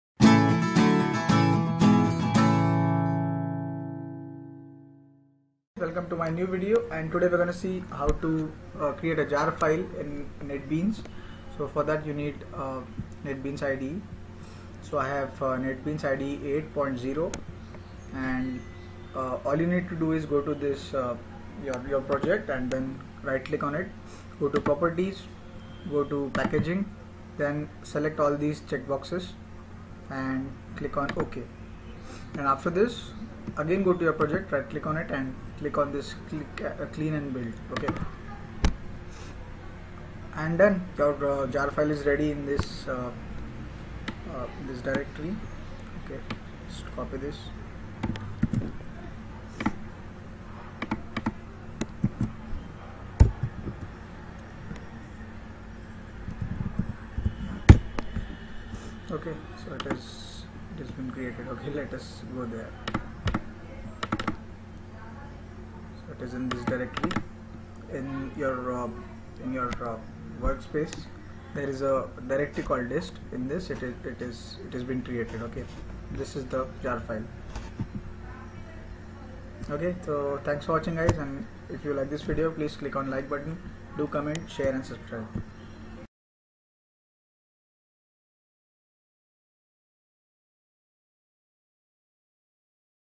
How to create Jar file in Netbeans in less than 2 mins.
This video is about how to create a jar file in Netbeans IDE.